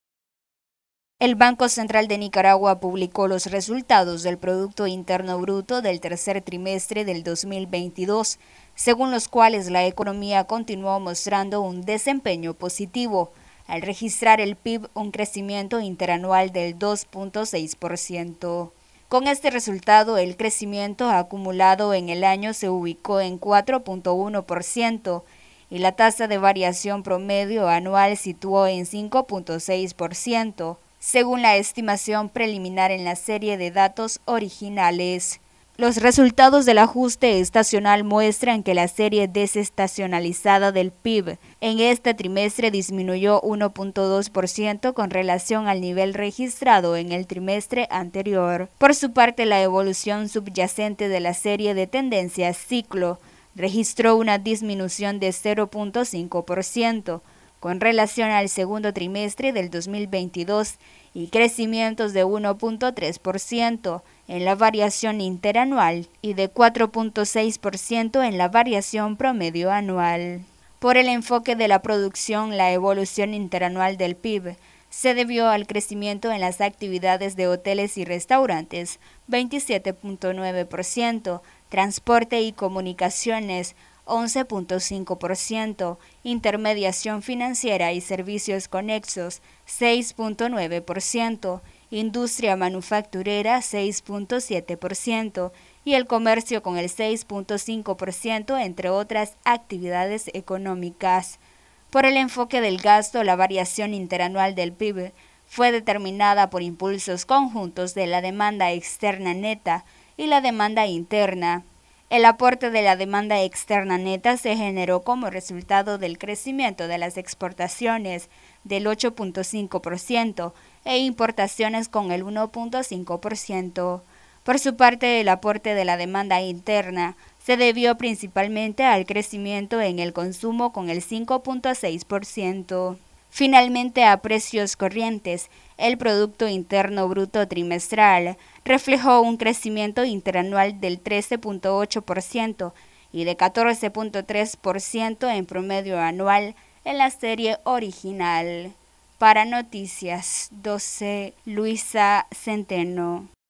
Noticias12Nicaragua PIB de Nicaragua registra un crecimiento interanual de 2.6 %, según BCN
Producto interno bruto de Nicaragua registra un crecimiento
interanual de 2.6 %, según BCN